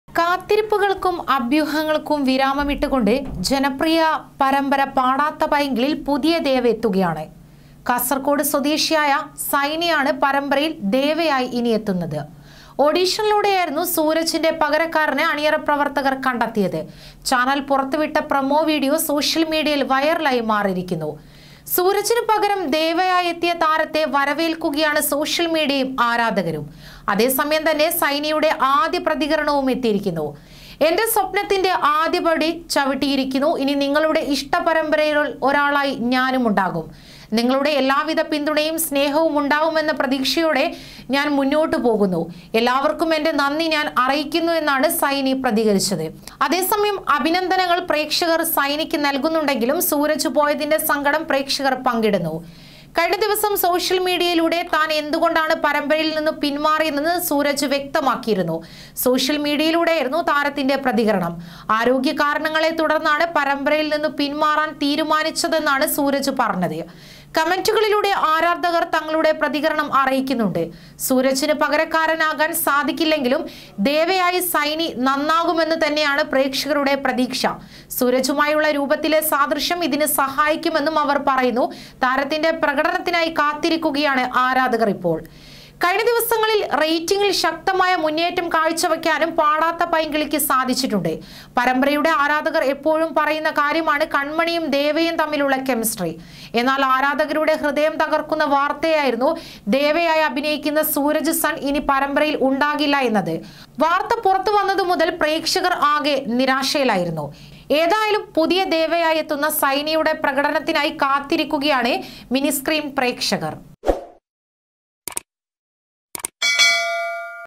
ഞങ്ങൾക്ക് ഇത് താങ്ങനാവുന്നില്ല; ഉൾക്കൊള്ളാനാകാതെ ആരാധകർ! | Paadatha Painkili
ദേവയായി എത്തുന്നത്
കാസർഗോഡുകാരൻ സൈനി
രൂപസാദൃശ്യം ചൂണ്ടിക്കാണിച്ച് പ്രേക്ഷകര്‍